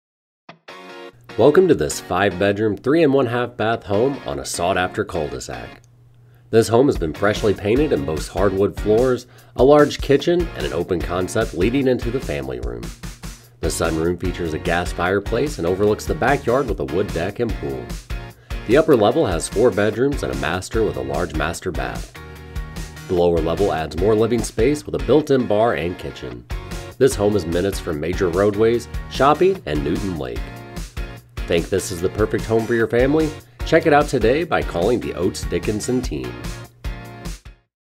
301 Kraft Court Haddon Township, NJ | ColdwellBankerHomes.com
301 Kraft Court, Haddon Township, NJ

Welcome home to this large five bedroom three and one-half bath contemporary home located on a sought-after cul-de-sac of Haddon Township. This home has been freshly painted throughout and features hardwood floors, a large kitchen with granite counter tops, an island, lots of cabinet and counter spa